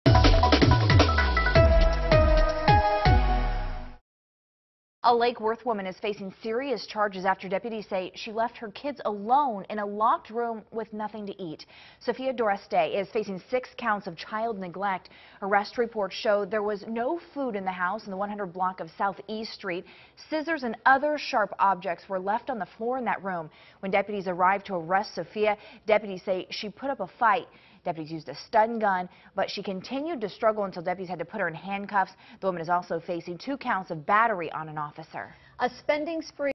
Deputies: Lake Worth woman left kids alone locked in room without food
A Lake Worth woman is facing serious charges after deputies say she left her kids alone locked in a room with nothing to eat. Sophia Doreste is facing six counts of child neglect.